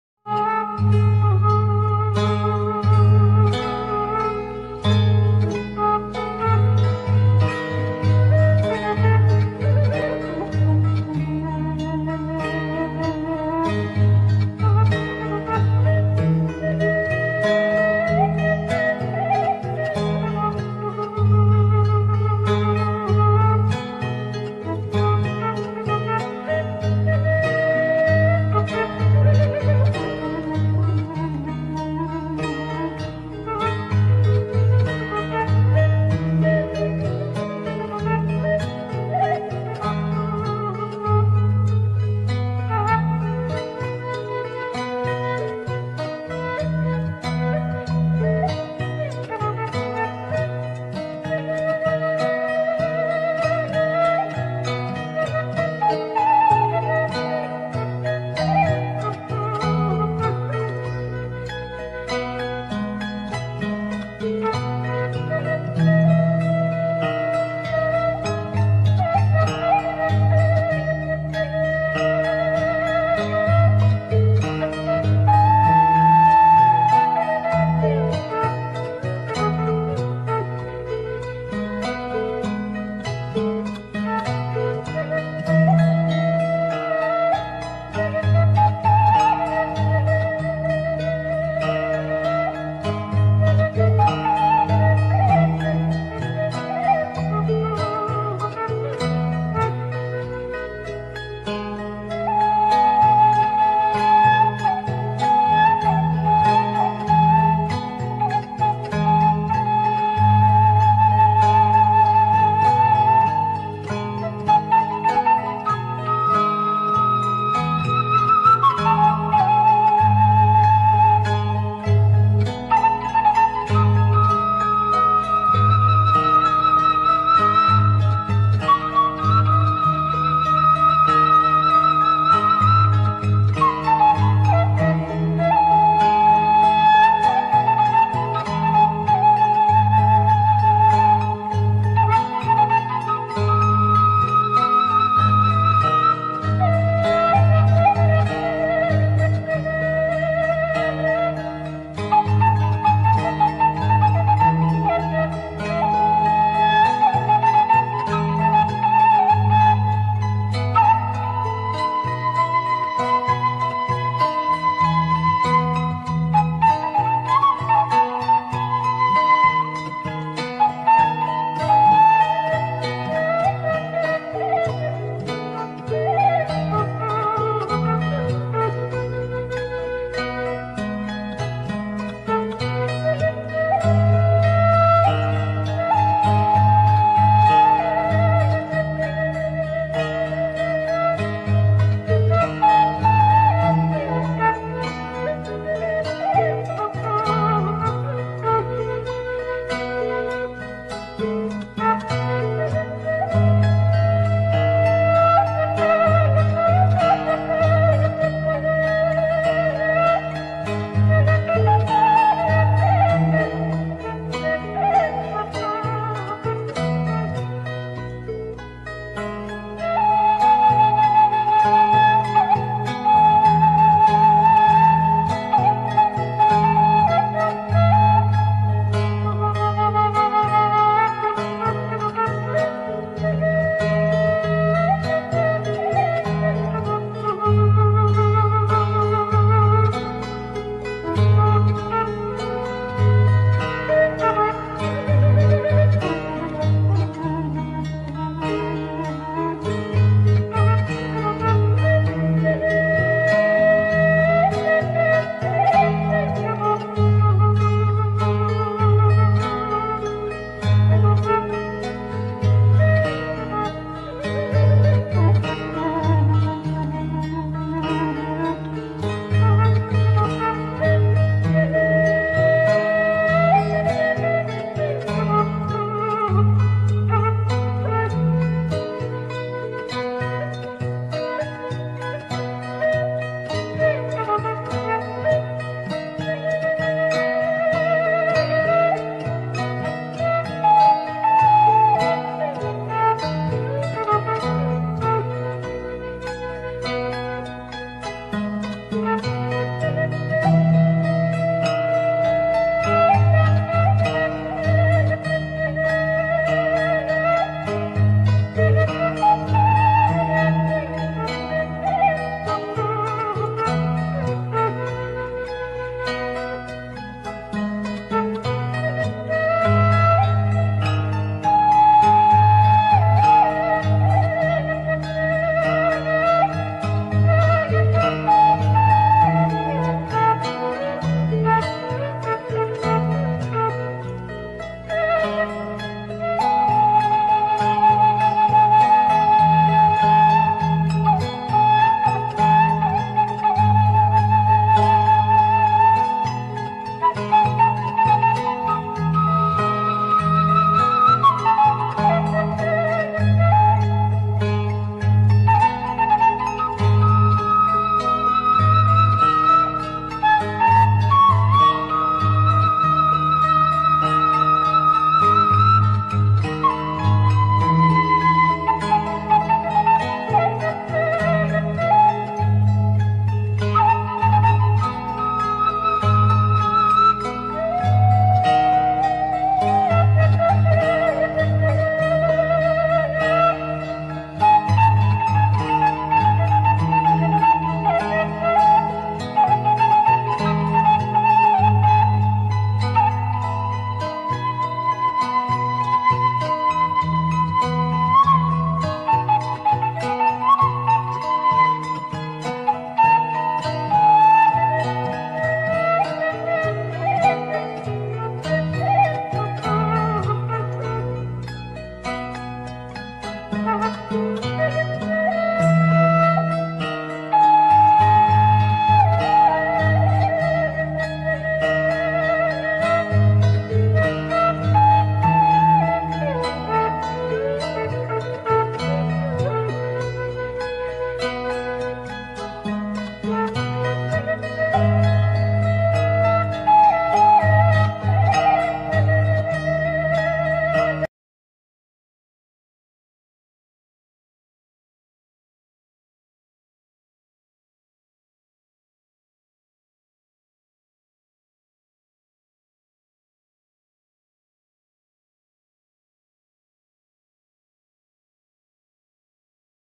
relaxation of flute music, Sundanese, West Java, Indonesia-rural nature
special relaxation music  relaxation of flute music, Sundanese, West Java, Indonesia-rural nature  for those who are tired and stressed after doing various kinds of activities, hopefully it can be a peaceful sedative